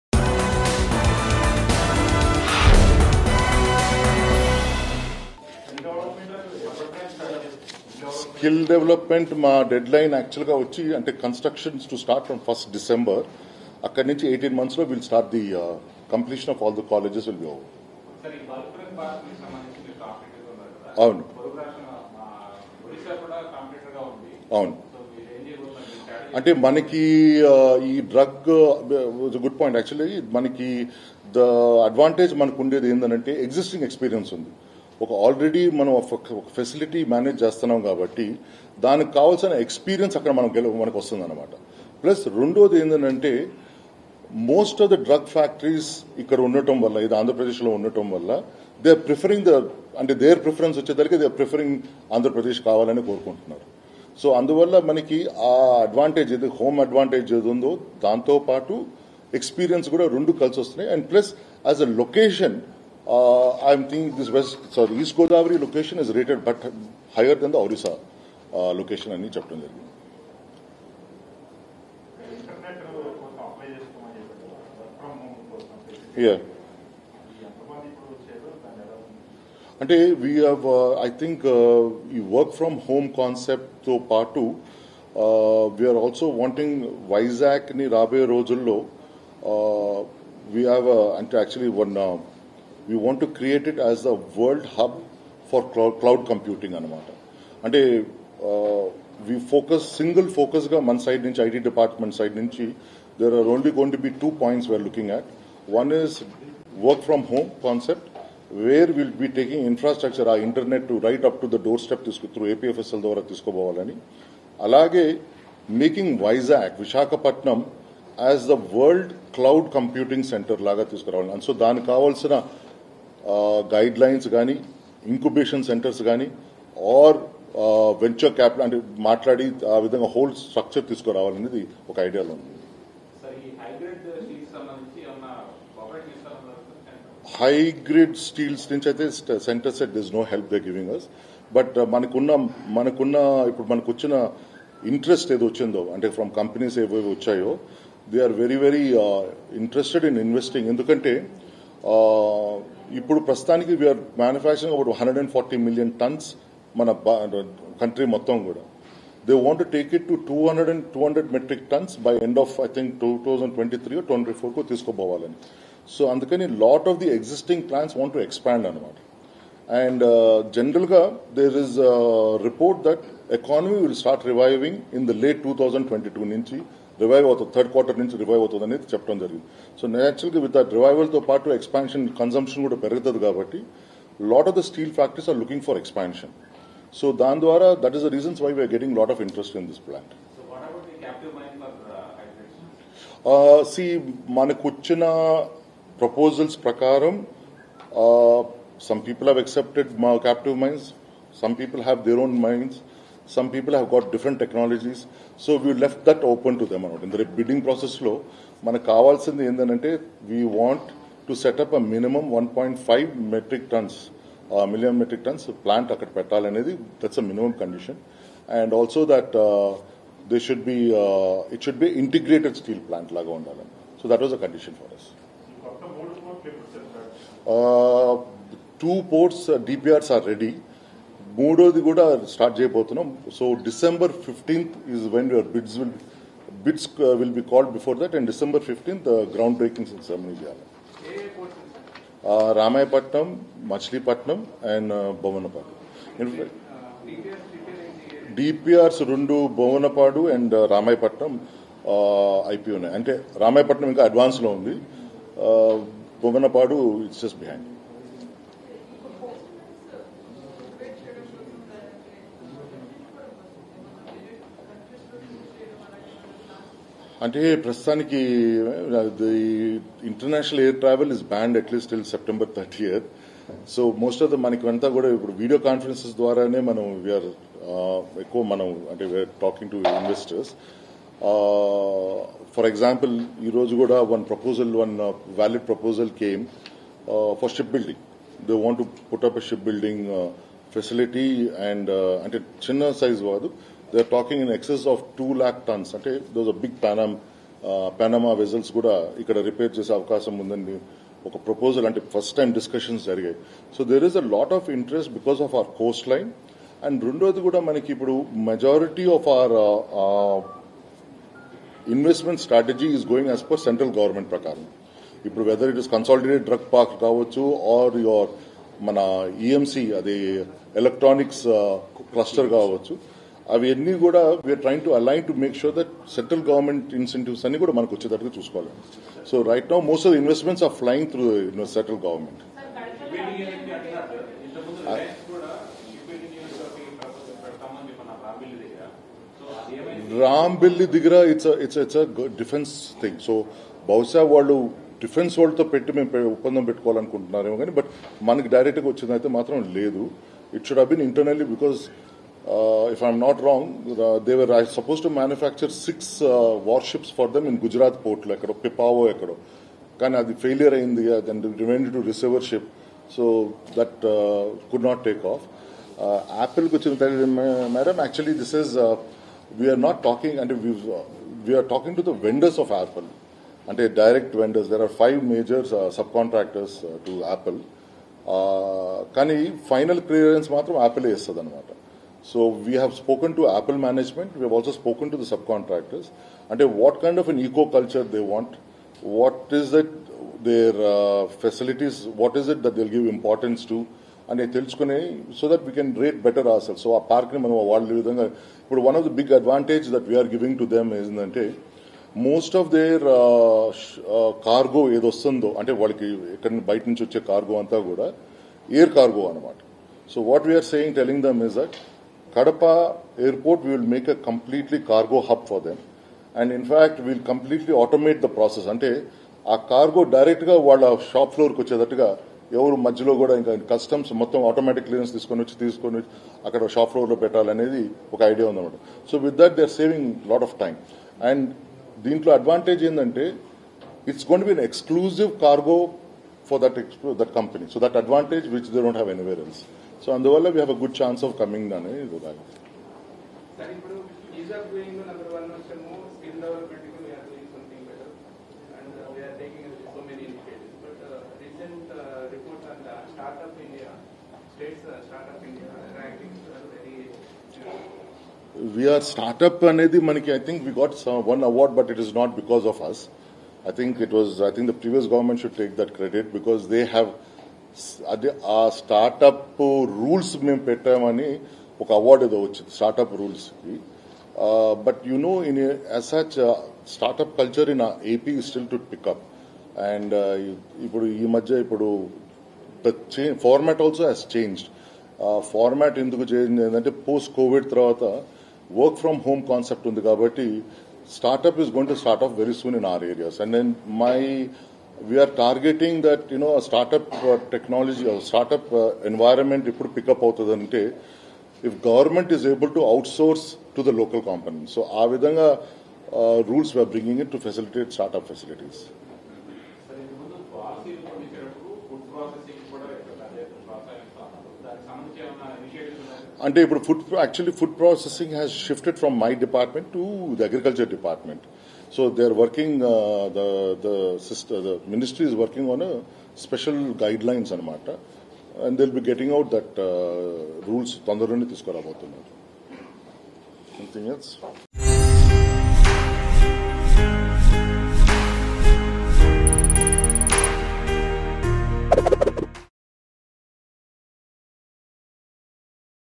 Minister Mekapati Goutham Reddy About Development Of IT Hub In Visakhapatnam | AP News | Mango News
Minister Mekapati Goutham Reddy About Development Of IT Hub In Visakhapatnam.